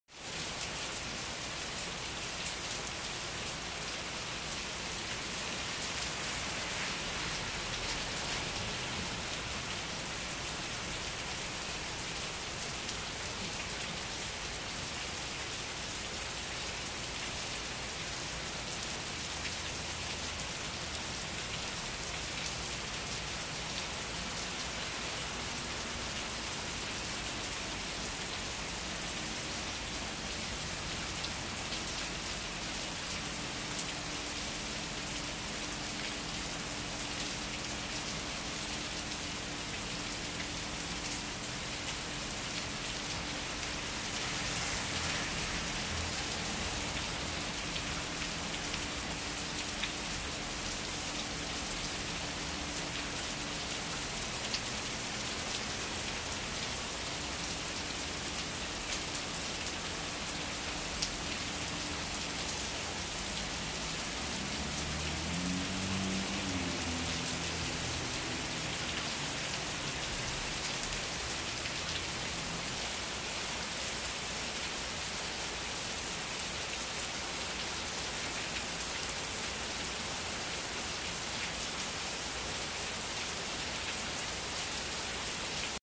Rainy Rolls Royce Ghost
Biturbo V-12 saloon, doesn't stand a ghost of a chance. Against monsoon rains. Just missed the Maserati Quattroporte on the Ave!